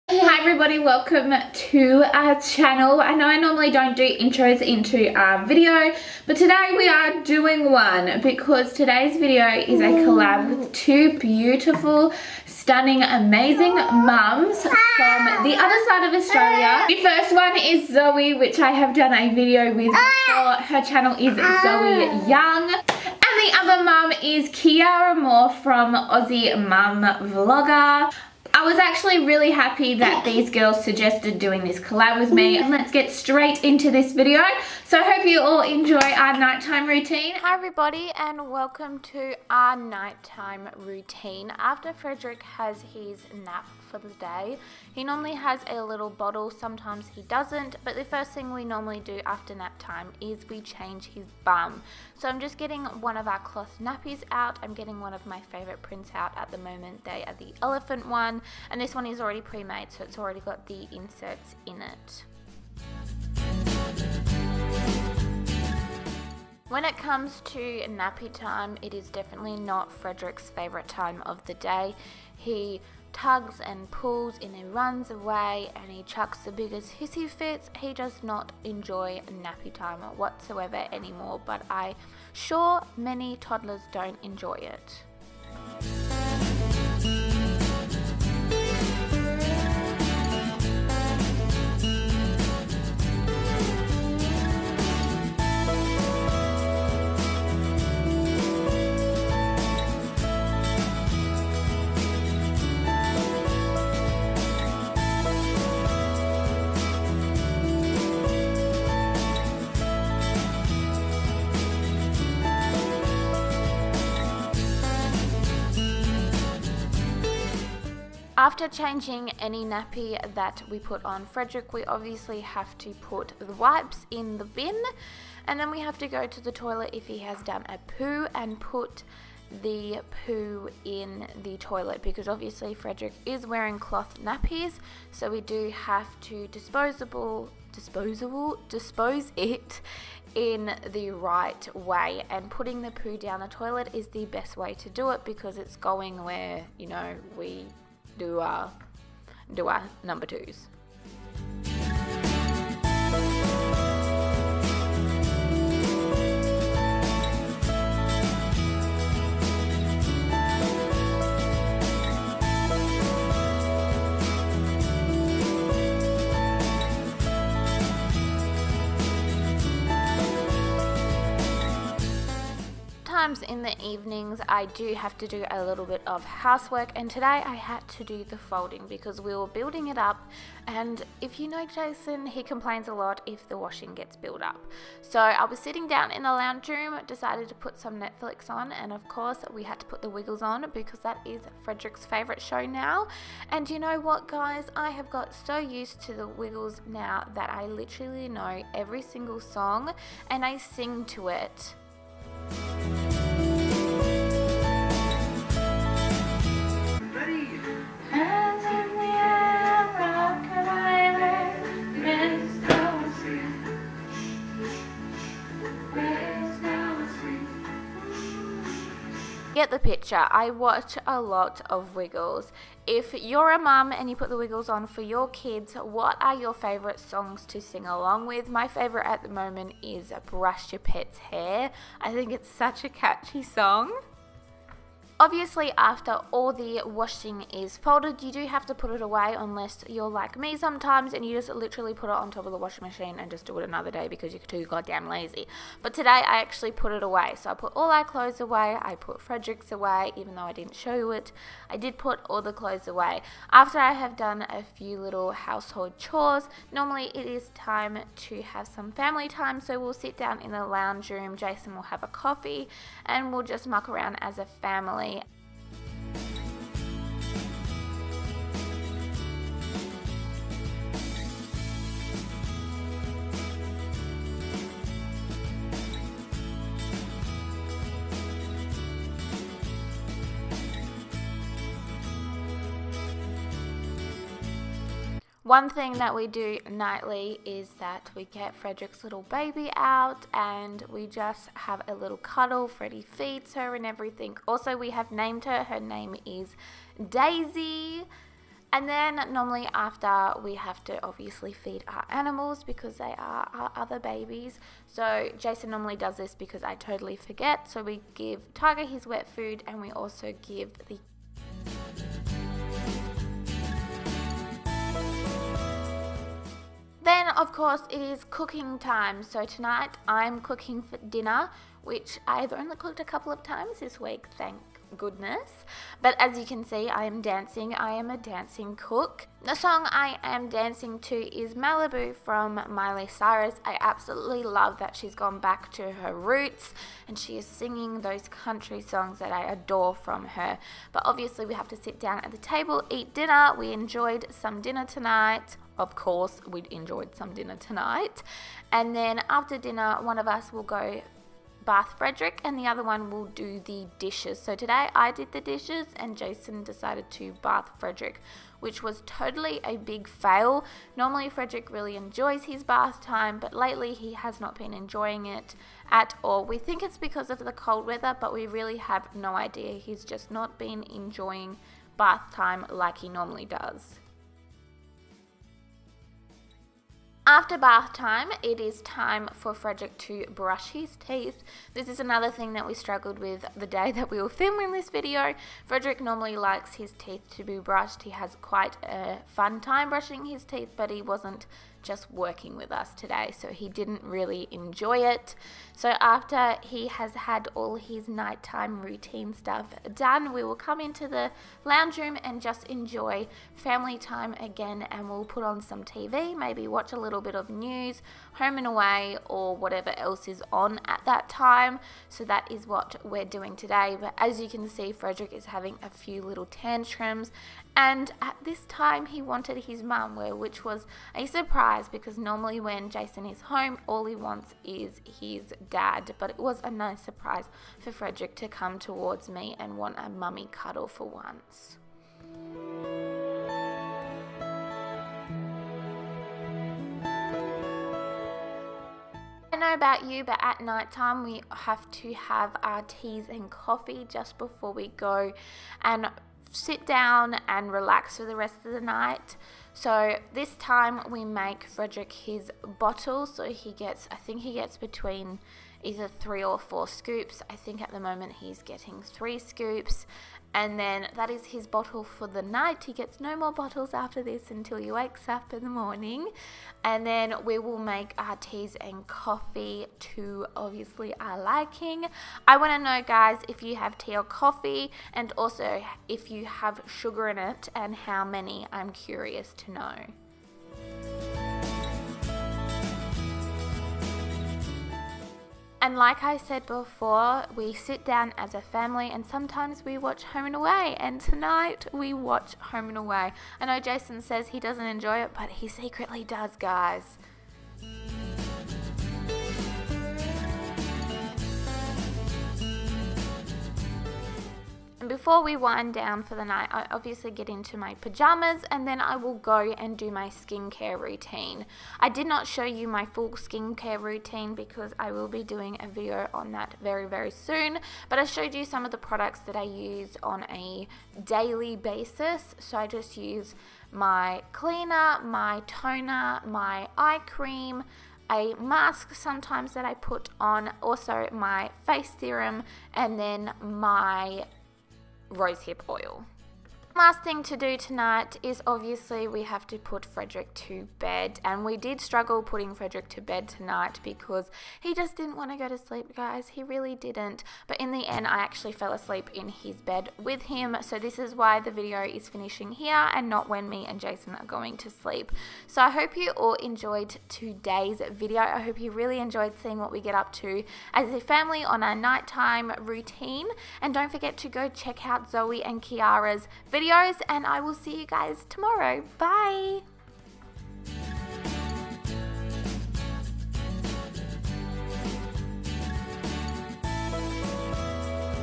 OUR NIGHTTIME ROUTINE | FAMILY BEDTIME
Tonight we are doing a very exciting video OUR NIGHTTIME ROUTINE! This is a collab with two beautiful bloggers Zoe and Keiara, they are both from Australia and both did a family bedtime routine.

Do you want to see a morning routine??

Warnbro WA
Australia 6169